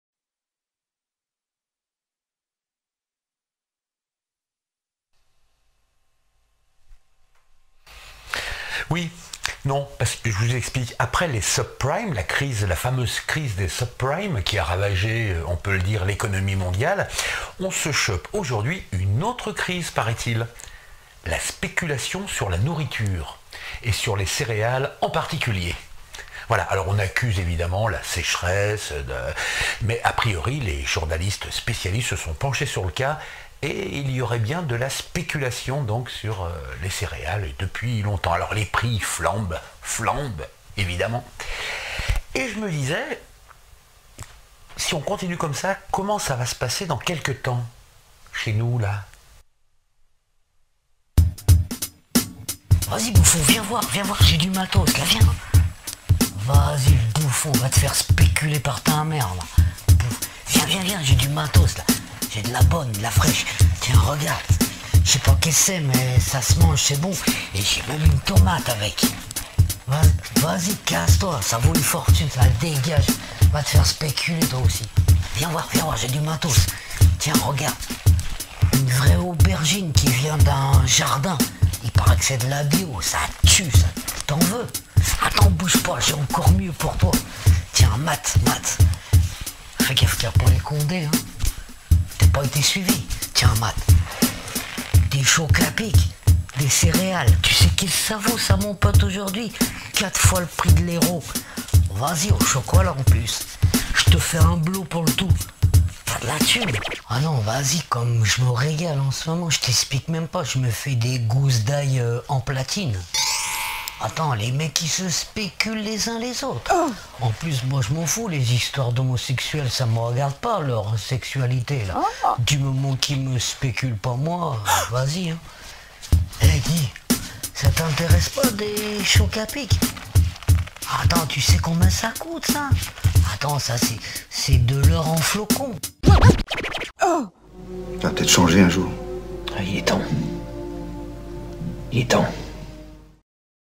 Spéculation sur la nourriture
Après les Subprimes, certains continuent a spéculer mais. sur la nourriture ! (source rue98.com)